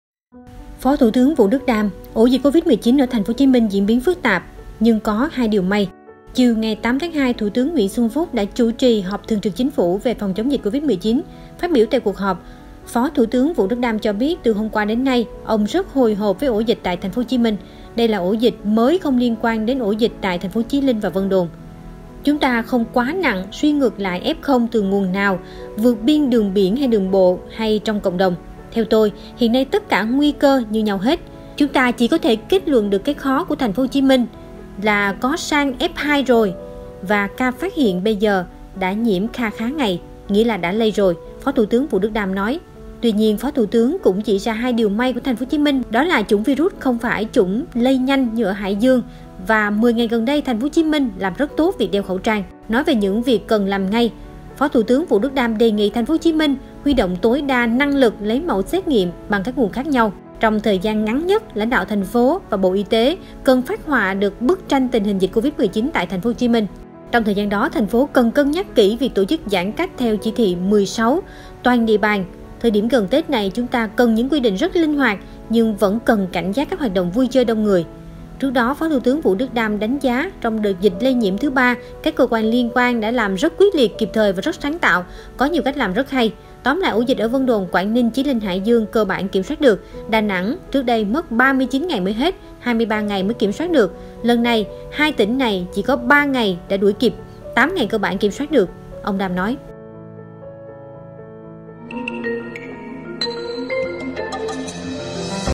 Phó Thủ Tướng bất ngờ công bố '2 ĐIỀU MAY' của TP HCM giữa tình hình dịch bệnh phức tạp
saigontvofficial Xem thêm thông tin và theo dõi chúng tôi tại